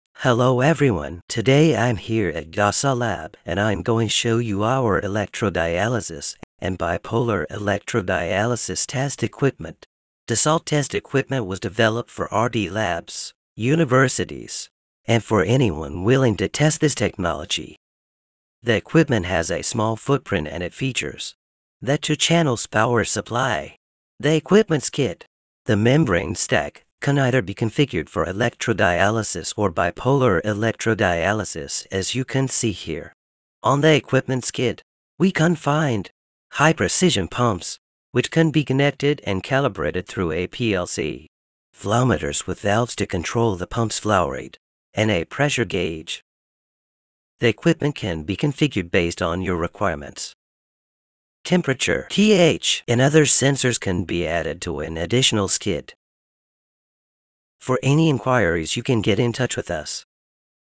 Electrodialysis & Bipolar Electrodialysis Test Equipment Presentation in 1 Minute - DESALT
💡 Thanks to its custom made design, DESALT Electrodialysis & Bipolar Electrodialysis lab test equipment can be used to test many different solutions and find the optimal configuration to scale up systems to industrial size projects.

🔎 In this short video we present our technology and describe its main advantages.

✔️ DESALT test equipment is currently used by universities and R&D lab all over the world.